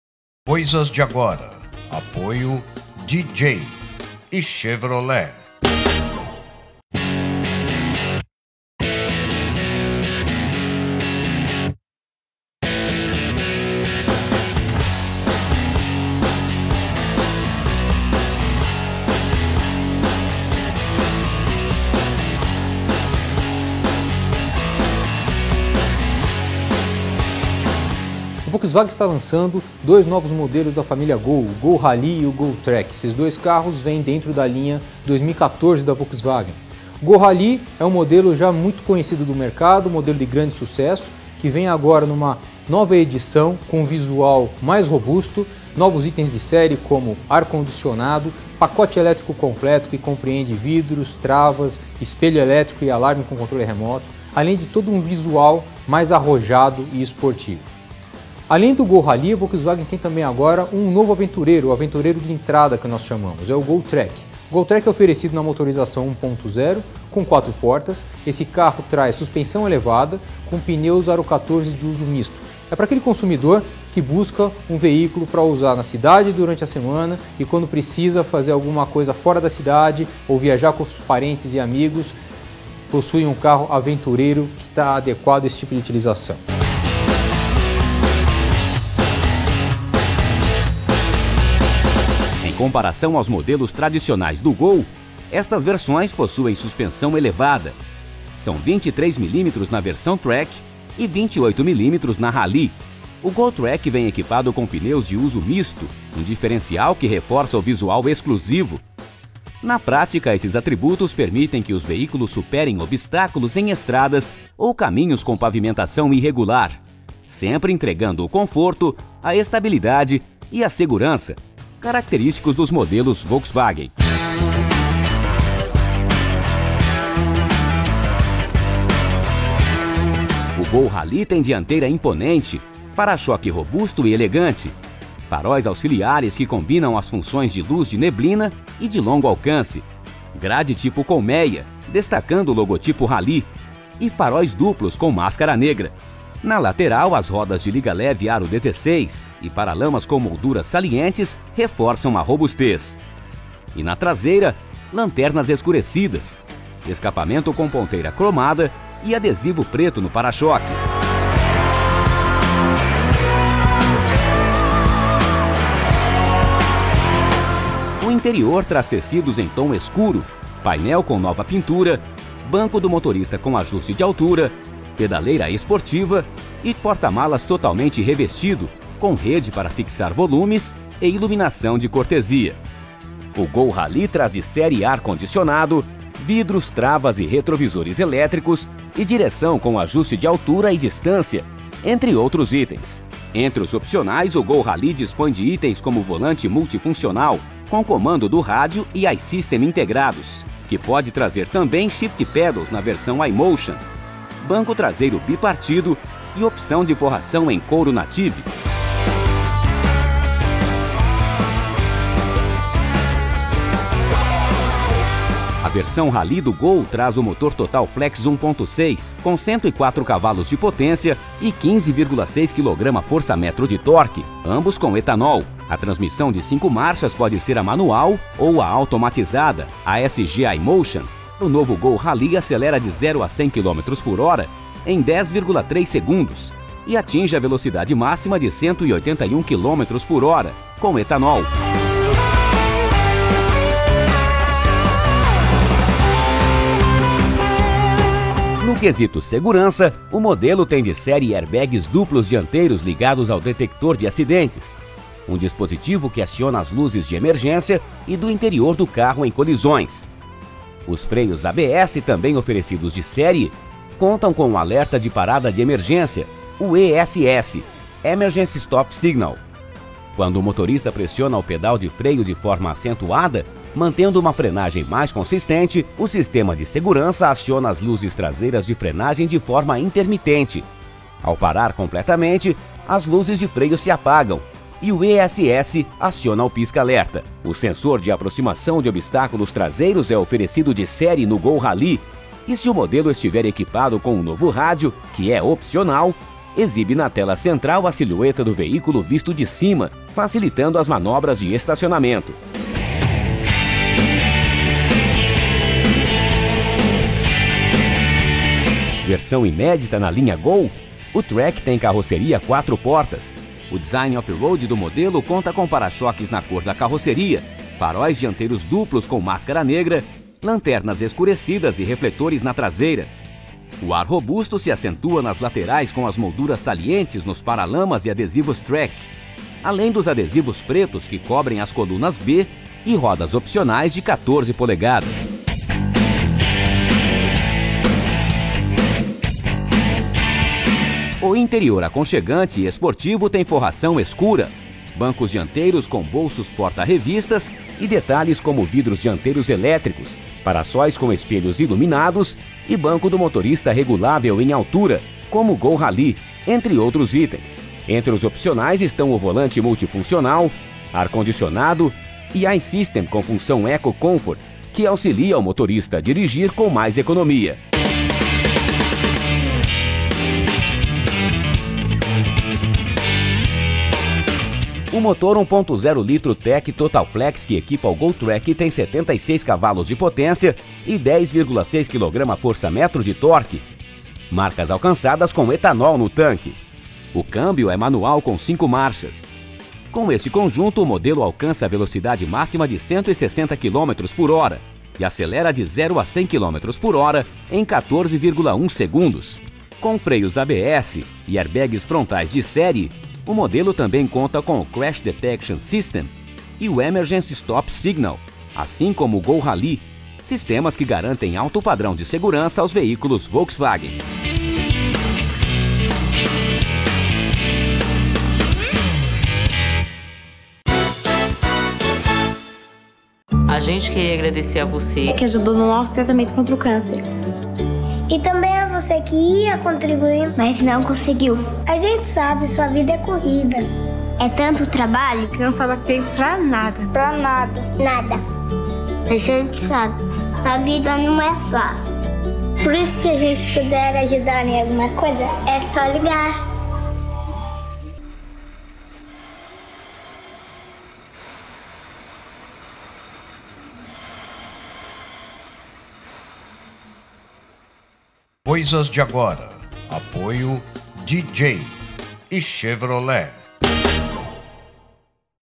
NOVOS GOL RALLYE E GOL TRACK
Coisas de Agora mostra o video release de lançamento do GOL Rallye e GOL Track 2014.